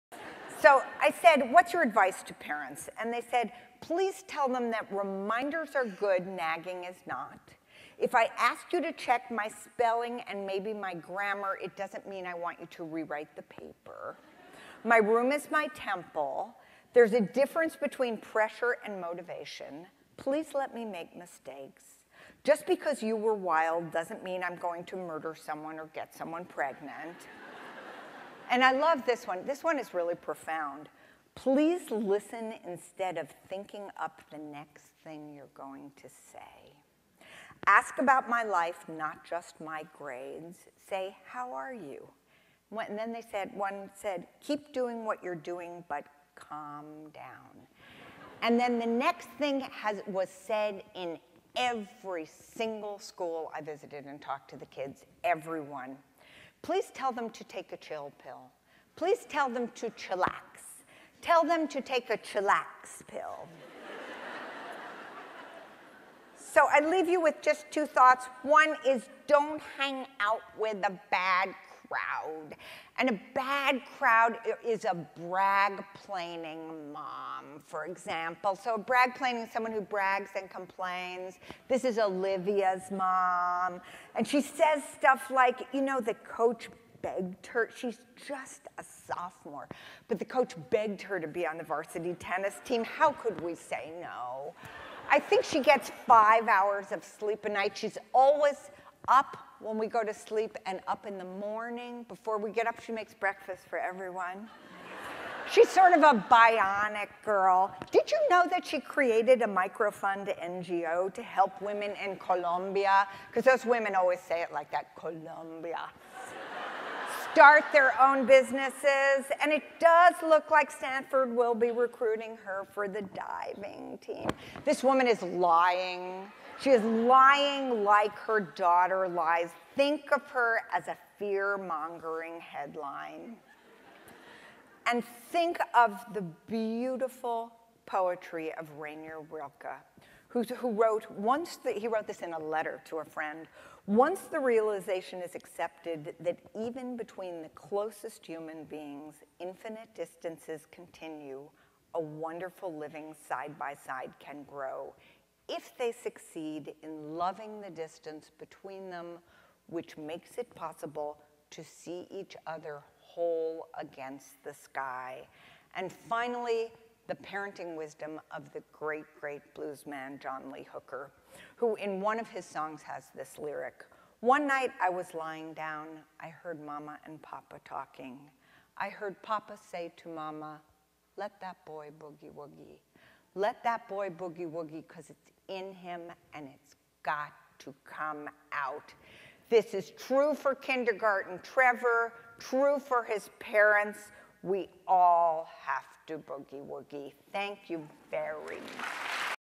Stanford Fall 2014 Conf 2 Keynote Excerpt Wendy Mogel
Wendy Mogel, Ph.D., is a
clinical psychologist and author of
the New York Times bestselling book,
'The Blessing of a Skinned Knee' and
follow-up book, 'The Blessing of a B Minus'.